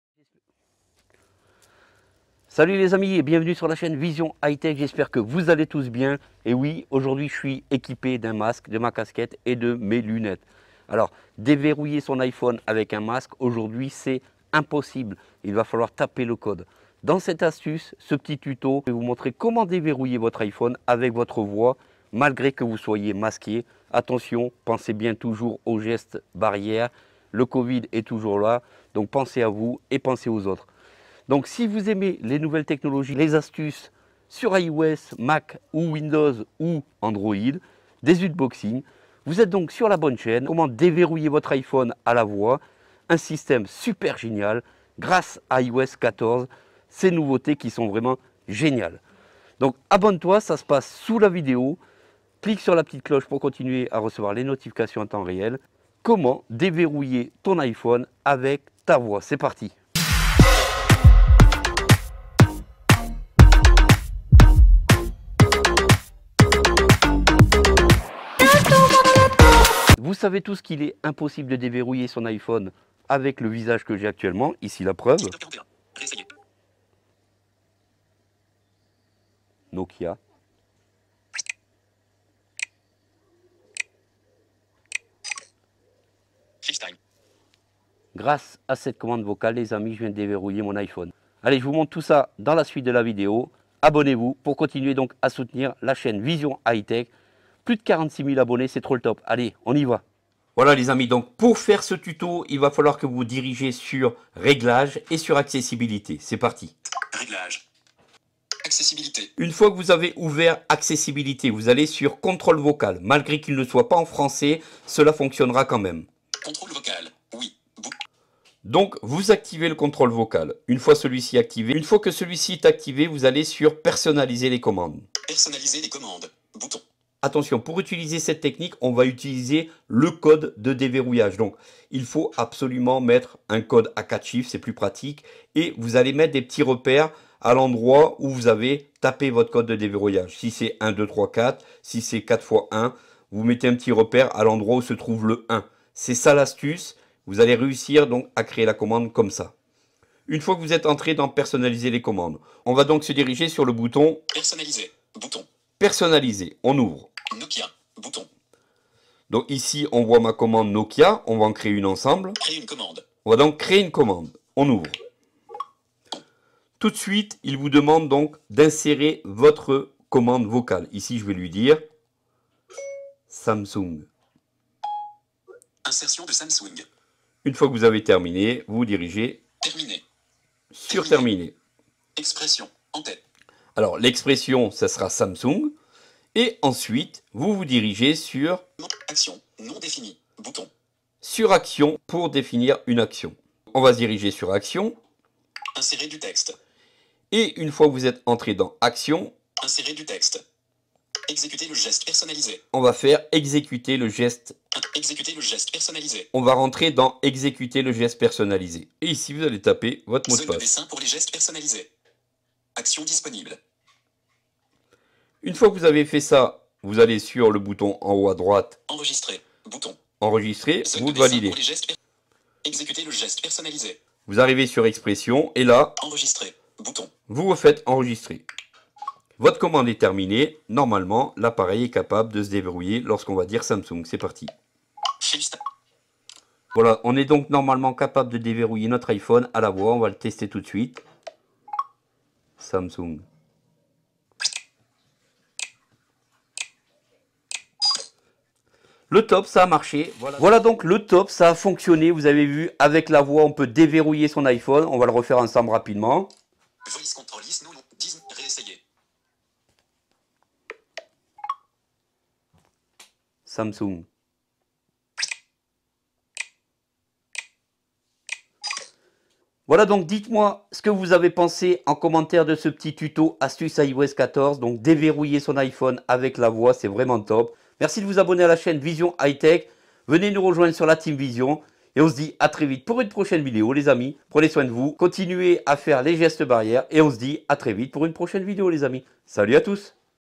iOS 14 iOS 13 Grosse Nouveauté Déverrouiller Votre iPhone Avec Votre Voix Même Masqué Face ID Vocal
Salut à tous grosse nouveauté sur iOS 14 attention même masqué ça marche. Astuce iOS 14 comment déverrouiller votre iPhone avec votre voix trop top, et tout ça encore grâce à l'accessibilité.Le Face ID vocal sous iOS 14 n'est plus de la fiction. Déverrouiller votre iPhone simplement par mot clé avec votre voix c'est vraiment génial et c'est sur Vision High-Tech.